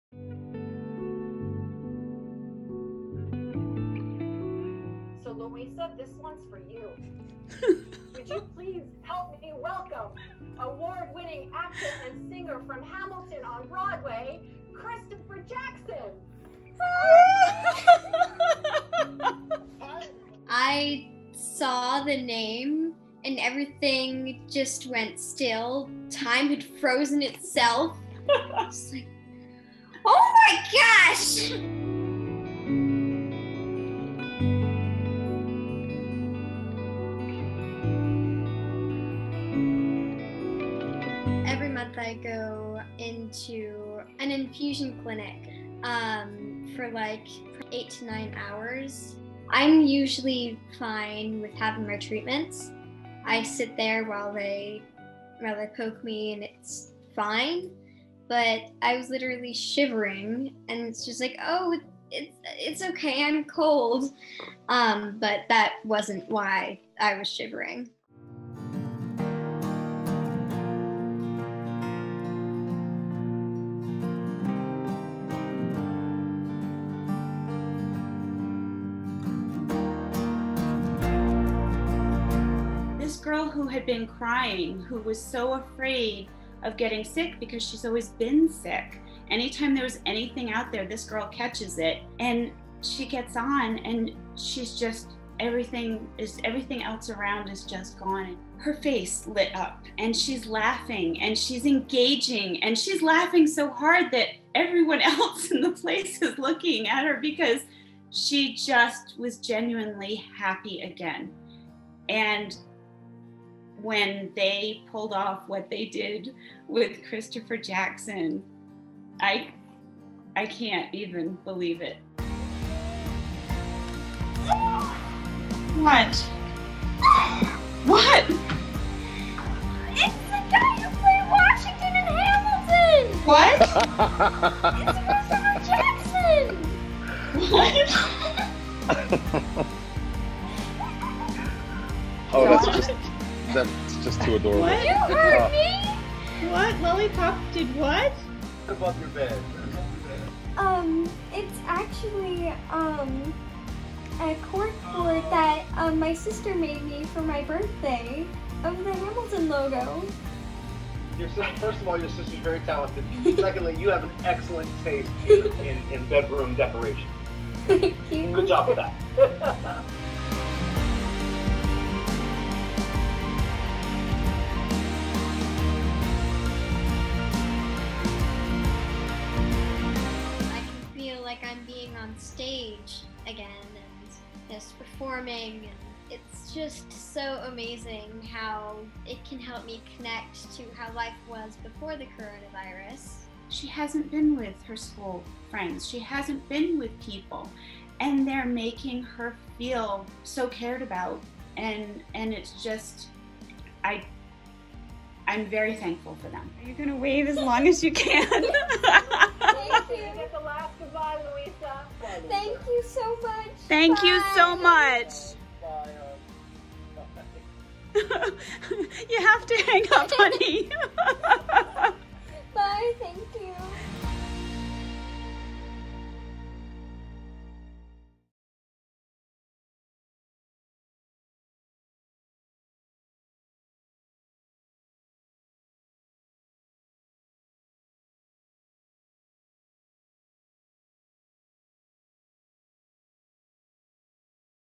'Hamilton' star surprises young fan on Zoom call | Humankind
12-year-old with common immune variable deficiency meets one of the stars of the Broadway show 'Hamilton.'